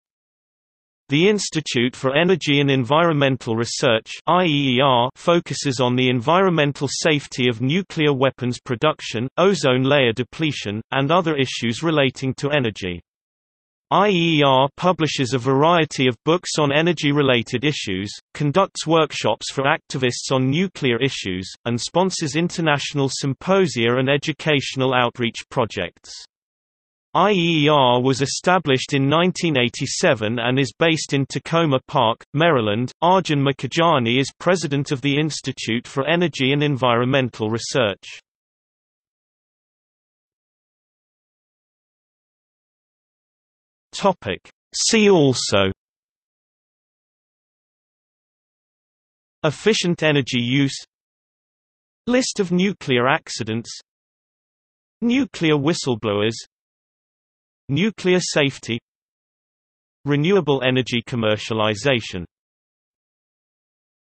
Institute for Energy and Environmental Research | Wikipedia audio article
00:00:40 See also

- Socrates

SUMMARY
The Institute for Energy and Environmental Research (IEER) focuses on the environmental safety of nuclear weapons production, ozone layer depletion, and other issues relating to energy. IEER publishes a variety of books on energy-related issues, conducts workshops for activists on nuclear issues, and sponsors international symposia and educational outreach projects. IEER was established in 1987 and is based in Takoma Park, Maryland.Arjun Makhijani is President of the Institute for Energy and Environmental Research.